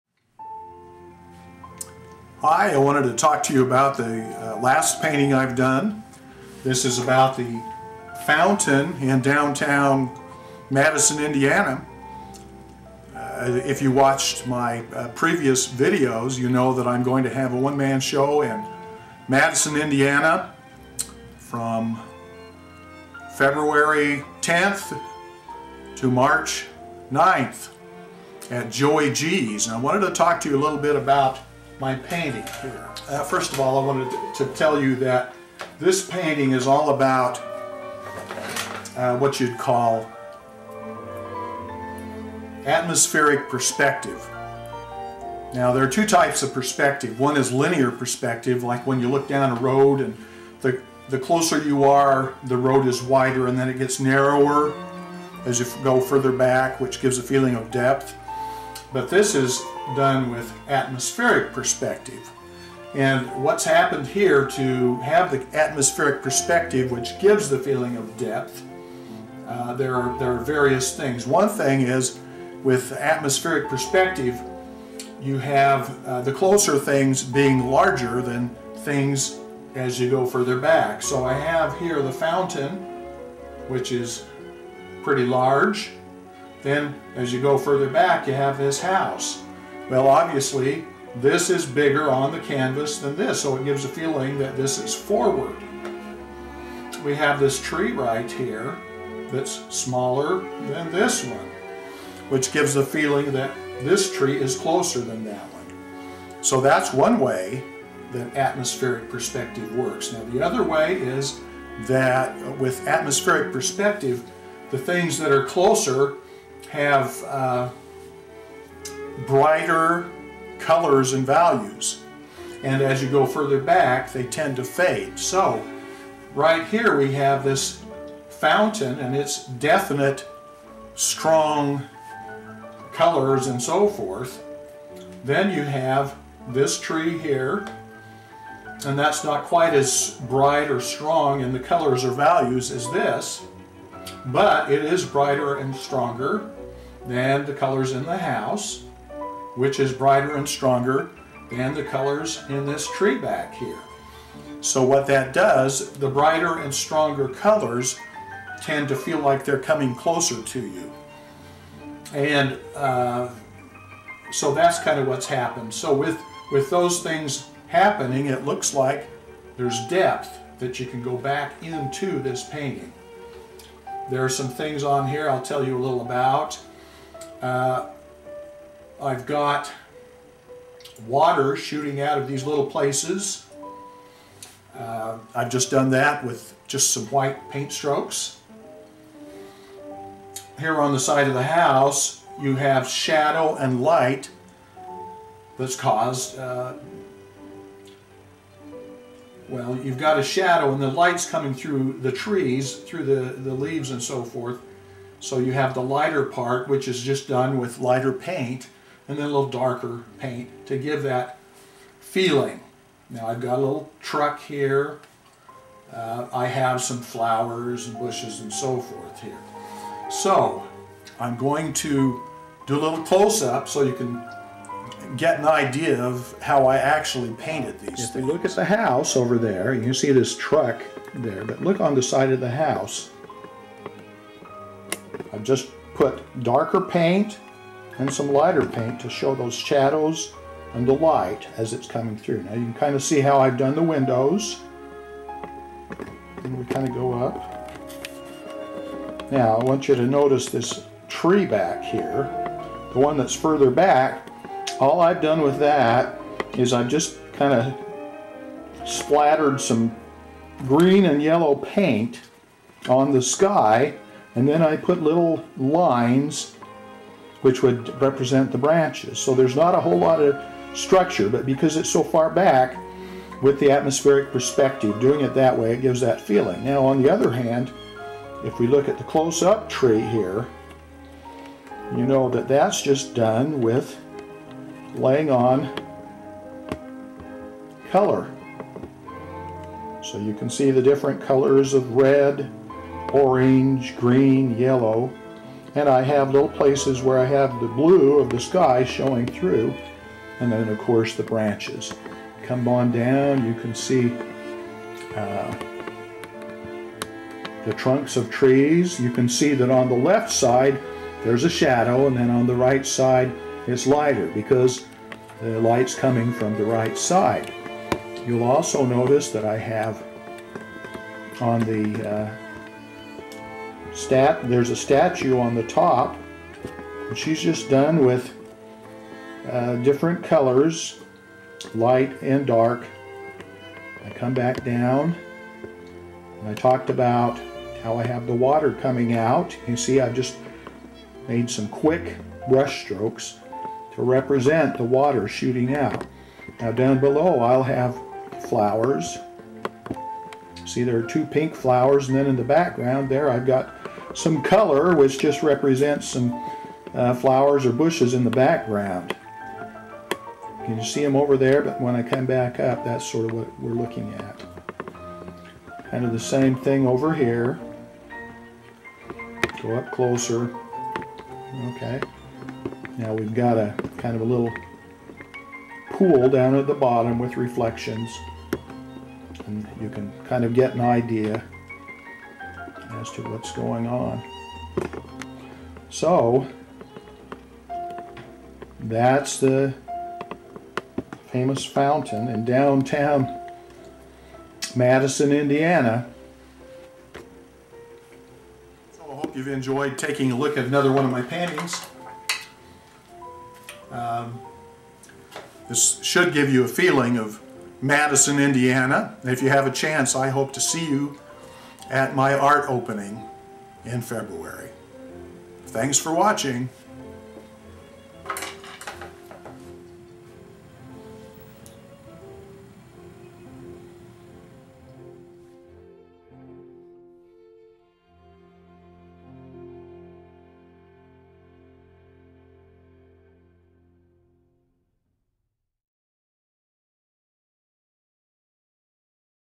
The Fountain
Tom Slack discusses his painting of the fountain in downtown Madison, Indiana.